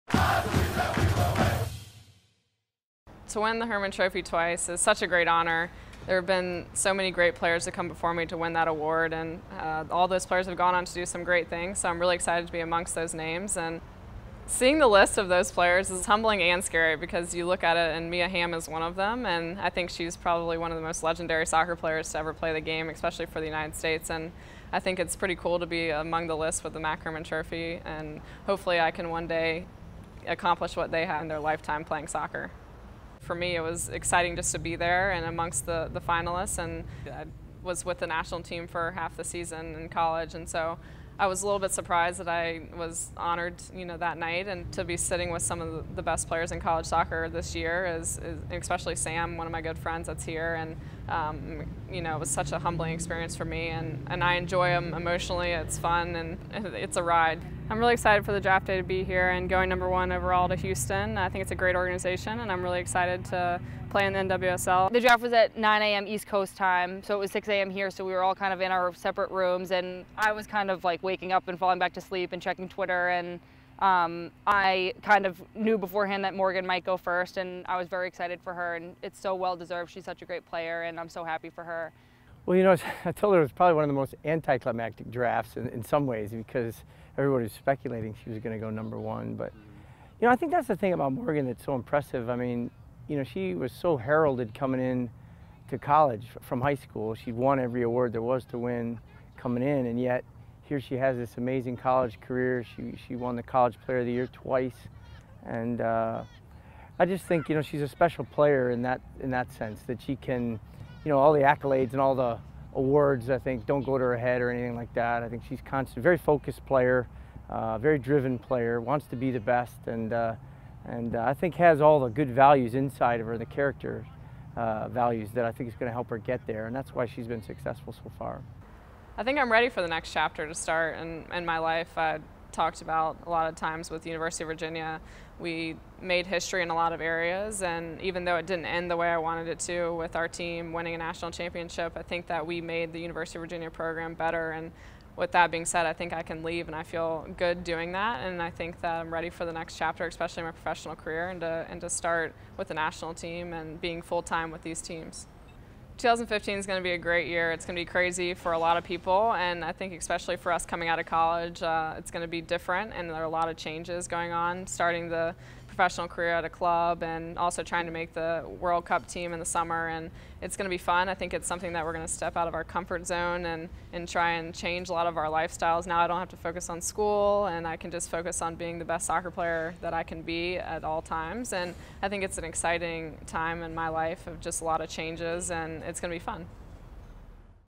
Morgan Brian’s Big Week
In the span of a week, young U.S. midfielder Morgan Brian won the MAC Hermann Trophy as college soccer’s top player – for the second consecutive year – and was taken as the #1 pick in the 2015 NWSL Draft by the Houston Dash. We hear from Brian on the whirlwind week and from U.S. WNT assistant coach and Brian’s head coach at Virginia, Steve Swanson, as well as from Samantha Mewis, her long-time teammate on the USA’s youth National Team, who went fourth overall in the draft to the Western New York Flash.

U.S. Soccer